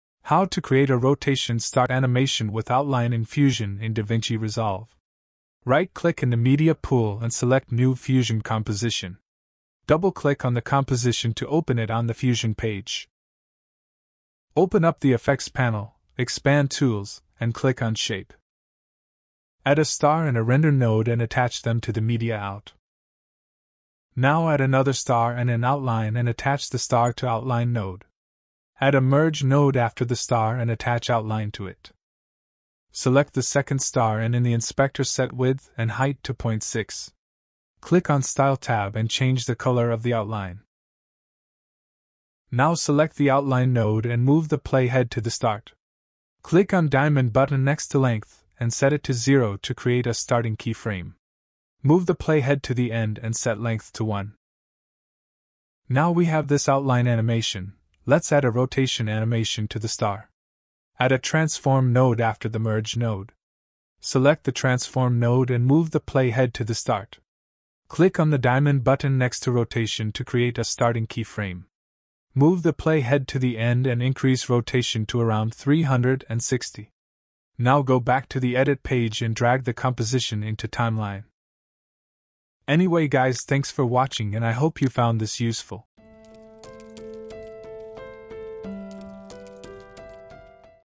How to Create a ROTATION STAR Animation with Outline in Fusion in Davinci Resolve
Easily create a dynamic rotation start animation with outlines in DaVinci Resolve's Fusion: Combine shapes, add outlines, and apply smooth rotation animations for visually engaging content.

How to Create a ROTATION STAR Animation with Outline in Fusion in Davinci Resolve Chapters:
00:00 create new fusion composition
00:14 add star shape in davinci resolve
00:24 add outlined star shape
00:43 animate outline in davinci resolve
01:01 animate start rotation